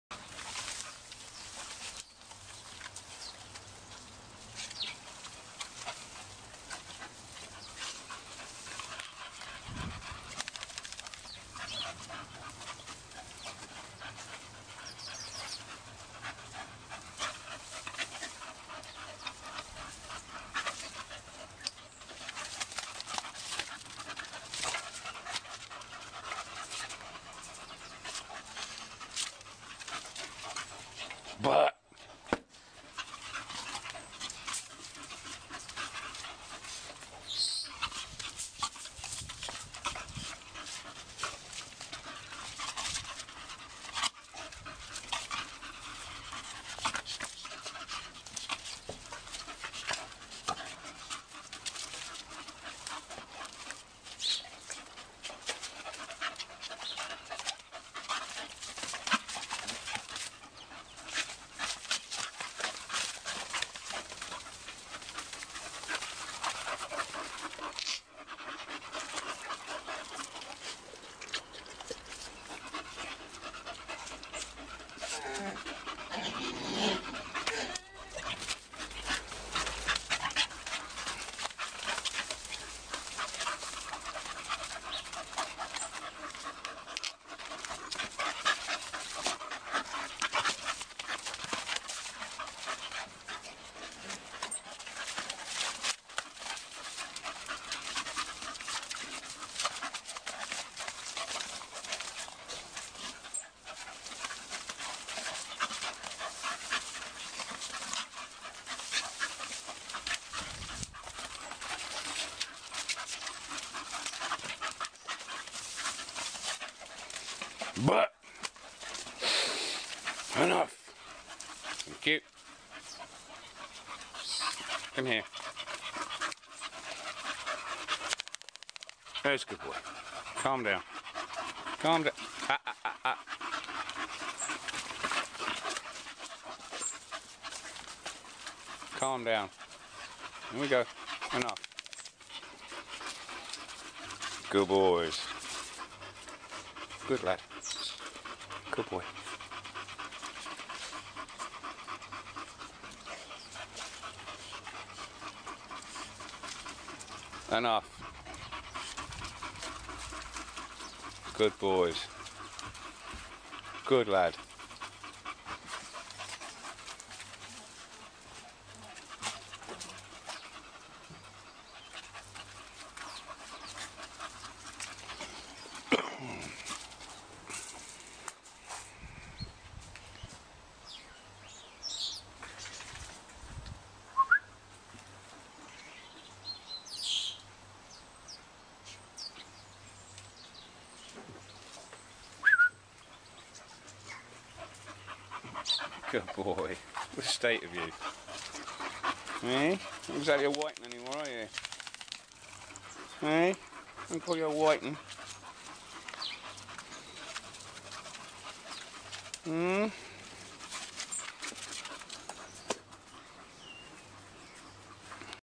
Enforce cut off point when it gets silly before they do with Bull Terriers
For new time Puppy Bull Terrier owners please watch and see why you do not let your puppy Bull Terrier run and play with others. They play very, very differently and most other breeds cannot and will not take it. Hence why you have problems. Socialize with other breeds on lead in controlled conditions. Do not think you can just go to local park and let junior off to play!! So do you really want a Bull Terrier puppy? Can you really put the time and effort in?